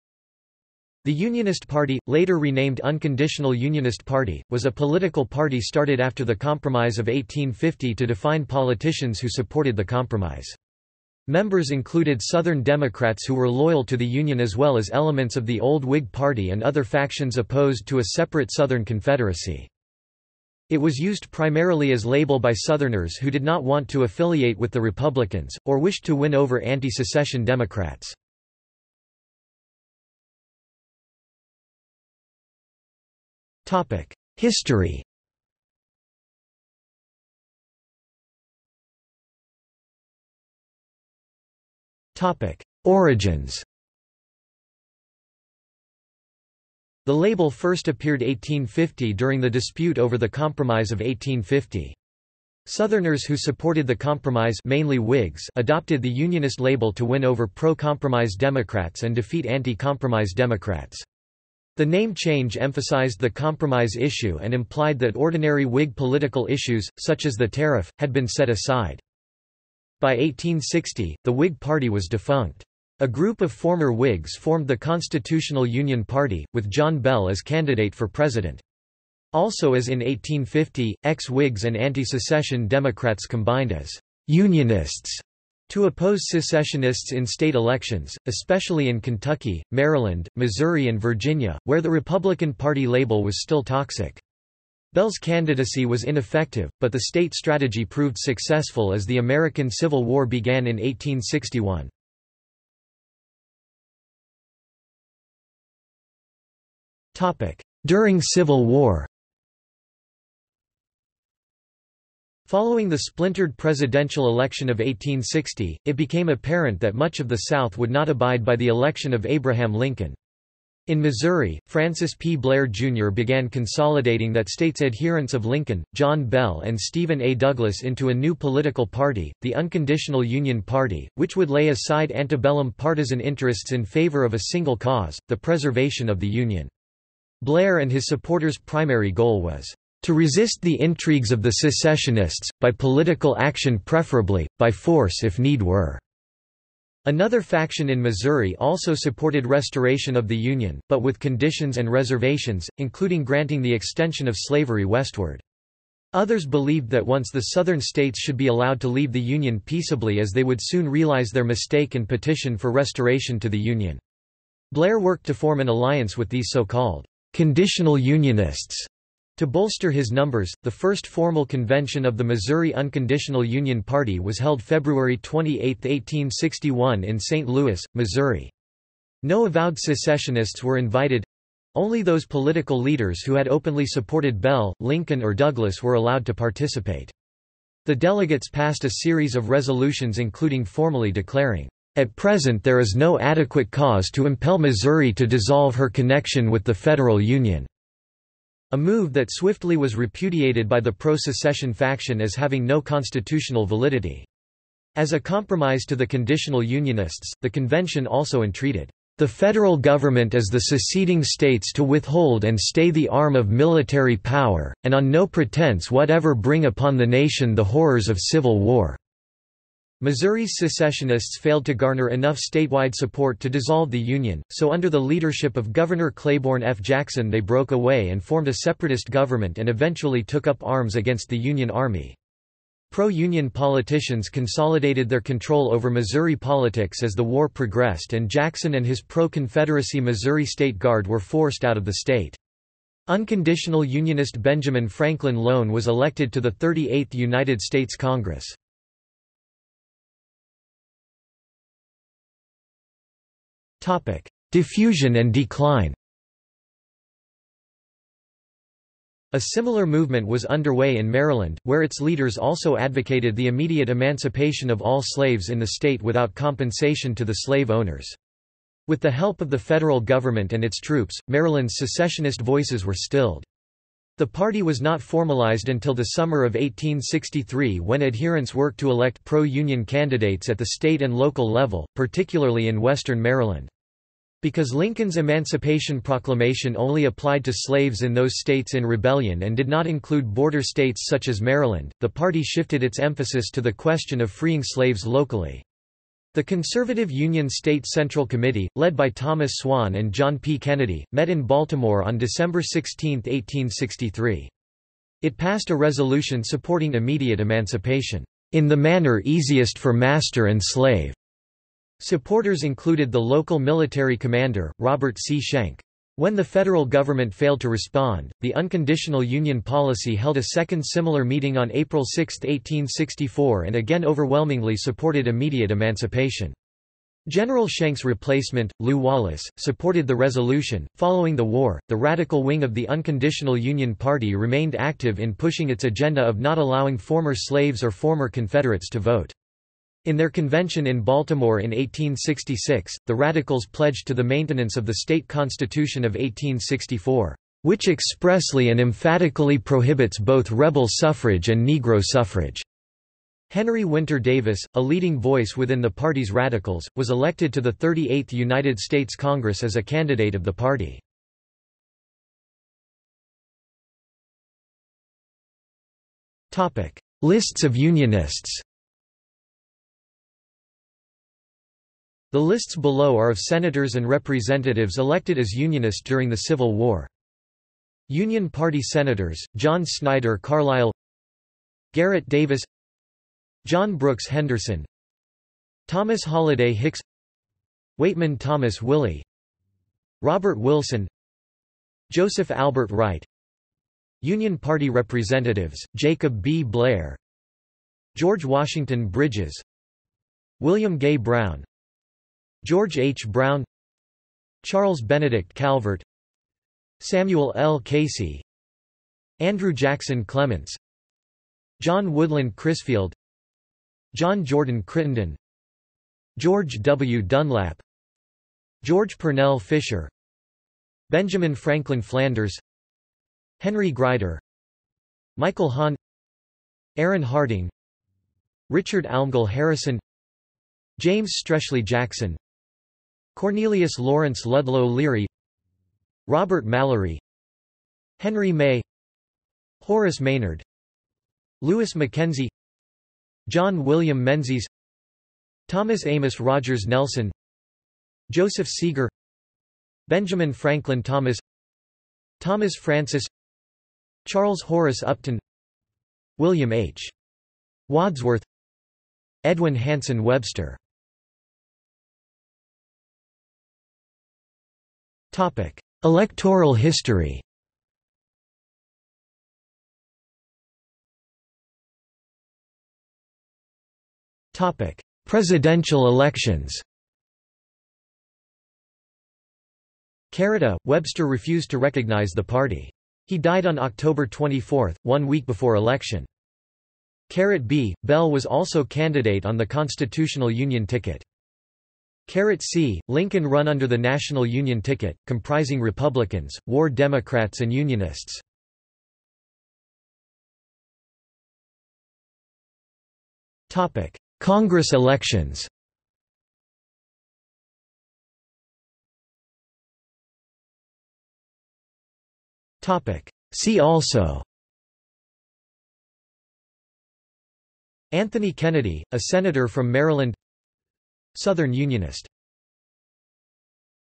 Unionist Party (United States) | Wikipedia audio article
This is an audio version of the Wikipedia Article:
Unionist Party (United States)

00:00:30 1 History
00:00:38 1.1 Origins
00:01:43 1.2 During Civil War
00:04:10 1.3 Diffusion and decline
00:06:02 2 Lists of Unionists
00:07:41 3 Electoral history
00:07:51 3.1 Presidential elections
00:08:20 3.2 Congress elections
00:08:29 4 See also

- Socrates

SUMMARY
The Unionist Party, later re-named Unconditional Unionist Party, was a political party started after the Compromise of 1850 to define politicians who supported the Compromise. Members included Southern Democrats who were loyal to the Union as well as elements of the old Whig Party and other factions opposed to a separate Southern Confederacy.